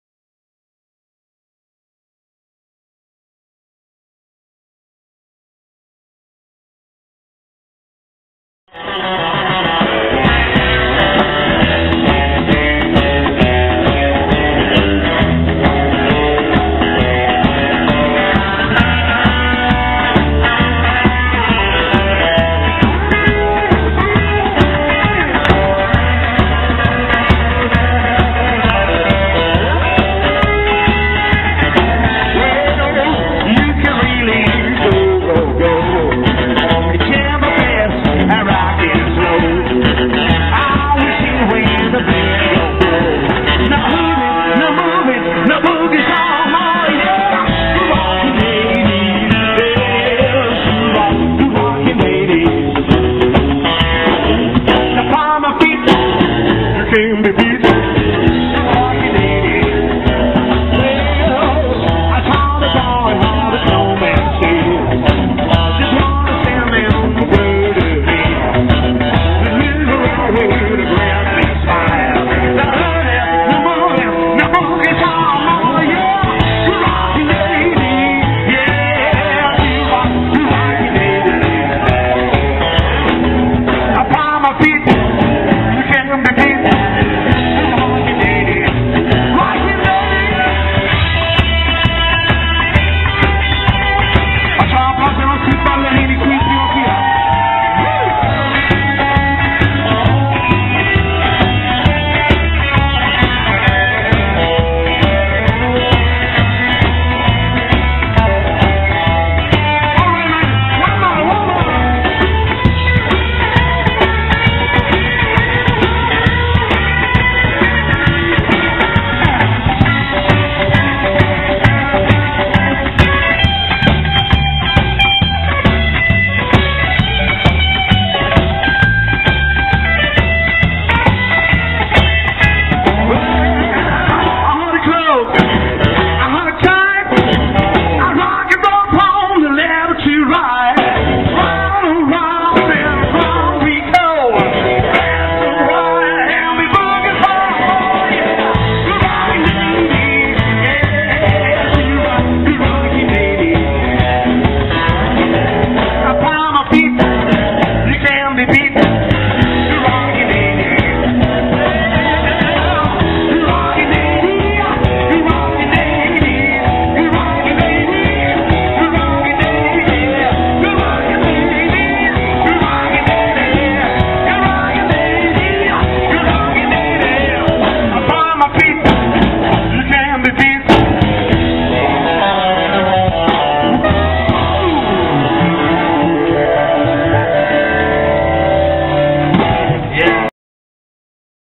Maveric Rock'n Roll Festival 2011 - Strike on the Beach
Gli Strike si esibiscono in mezzo alla gente sul lungomare di Crotone durante il Maveric...